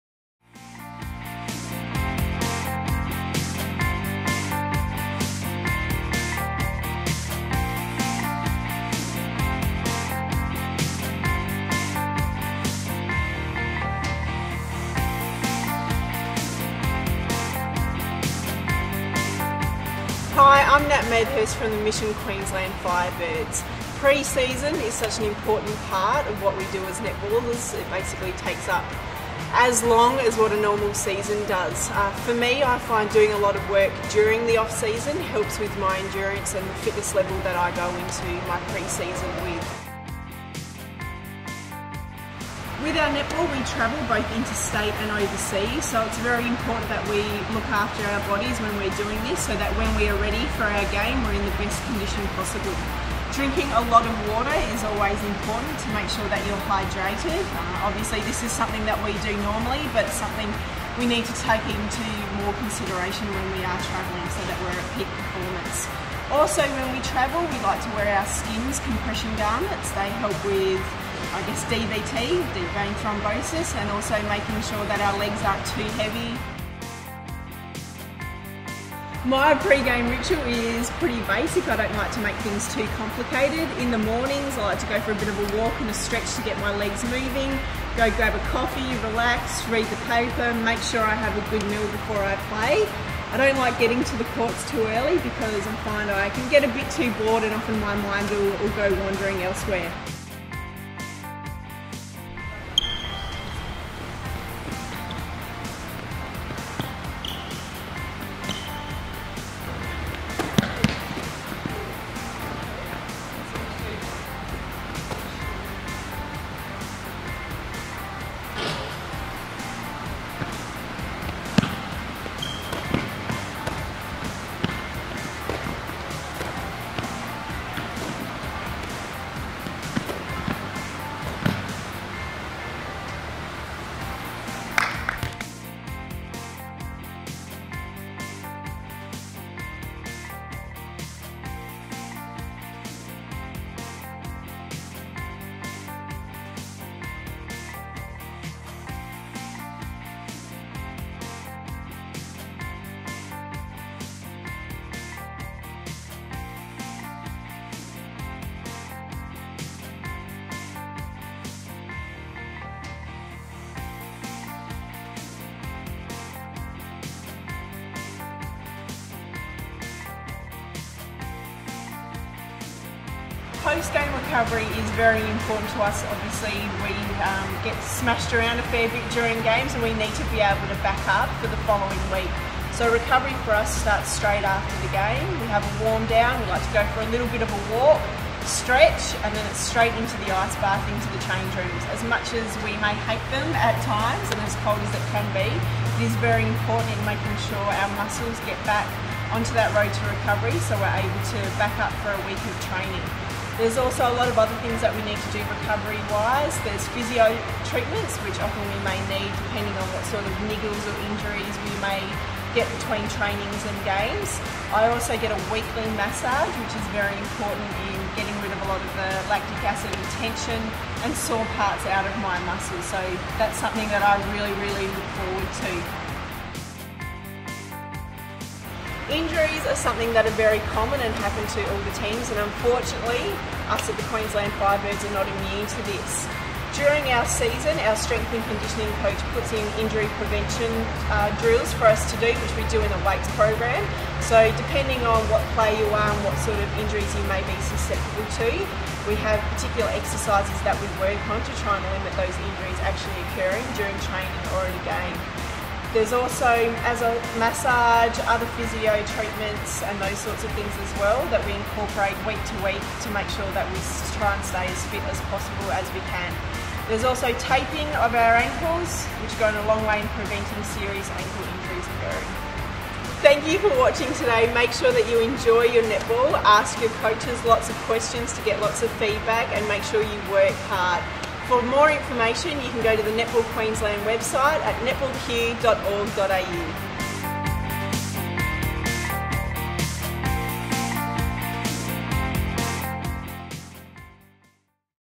Firebirds Skills Video: Natalie Medhurst
Watch as Natalie Medhurst demonstrates Agility Footwork for Shooters and Shooter Rotation. Proudly supported by the Queensland Government.